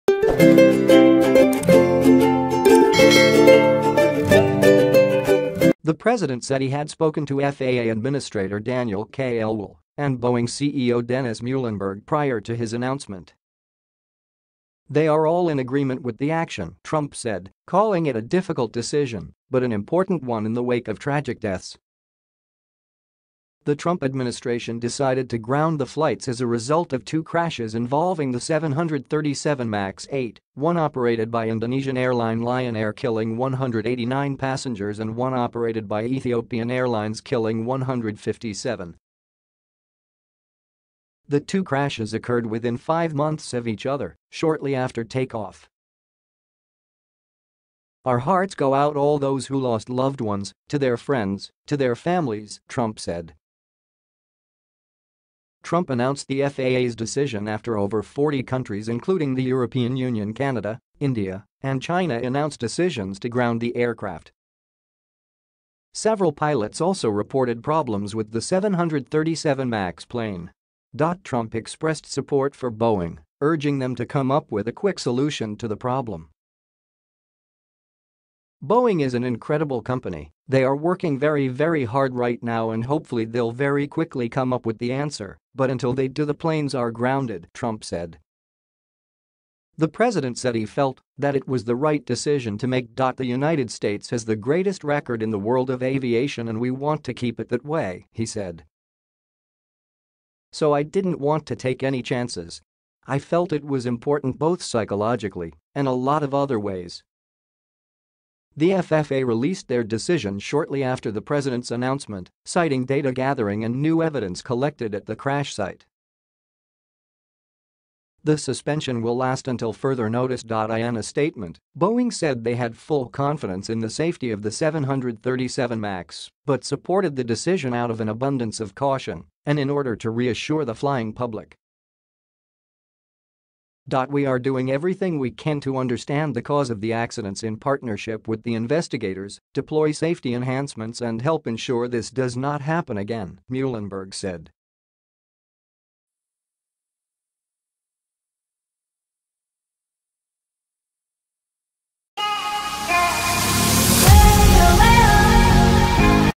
Donald Trump announced plans to ground Boeing 737 Max 8 and 9 planes
The president said he had spoken to FAA administrator Daniel K. Elwell and Boeing CEO Dennis Muilenburg prior to his announcement. “They are all in agreement with the action,” Trump said, calling it a